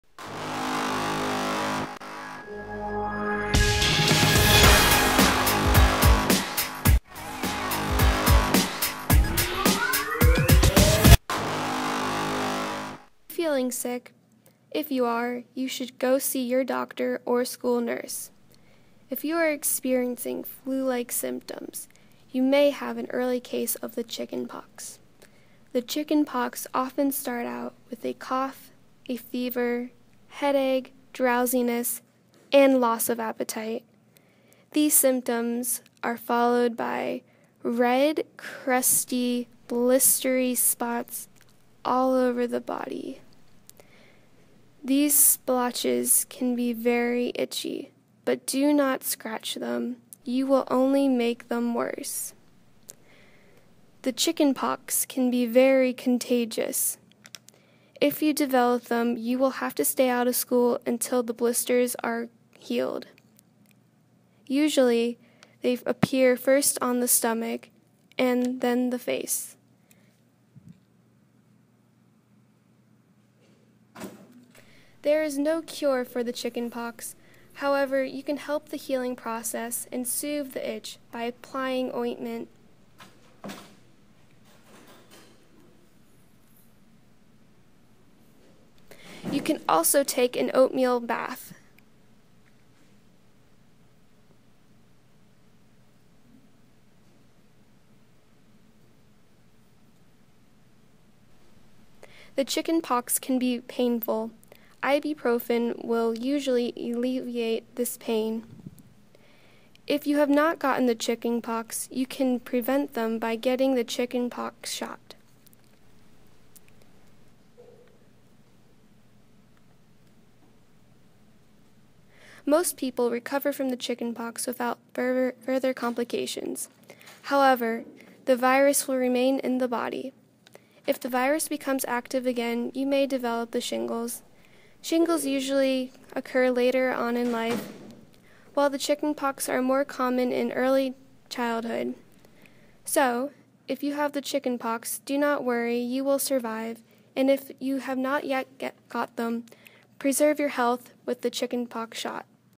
Chicken Pox Symptoms, Recovery, and Prognosis
This is a project that I had to do for my Human Anatomy class. My and my friend put together and educational PSA about the Chicken Pox. If you think you are feeling sick and have a rash, this video might help you diagnose yourself (but I really reccomend a doctor!). If you currently have the Chicken Pox, our video will give you a few remedies for the itching and pain as well as provide you with a prognosis. I hope this is helpful and that you enjoy it!!! xoxo